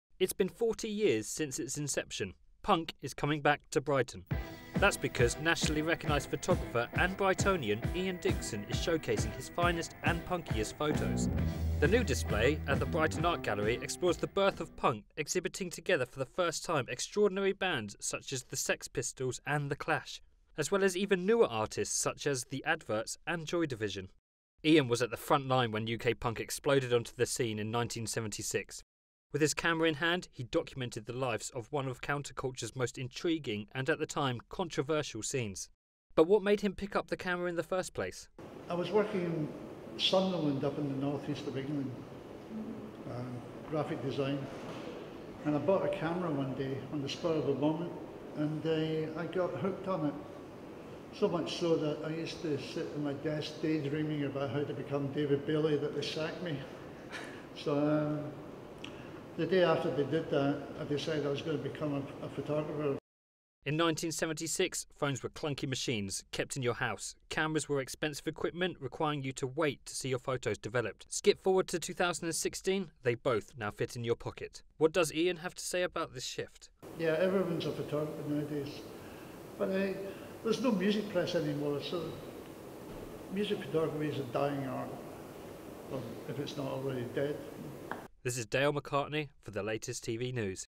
40 images from the birth of punk by Ian Dickson
40 years on from it's birth - the punk scene is returning to Brighton as photography exhibition opens.. featuring some of Ian Dickinson's finest work.

Latest TV's Dale Mccartney has more..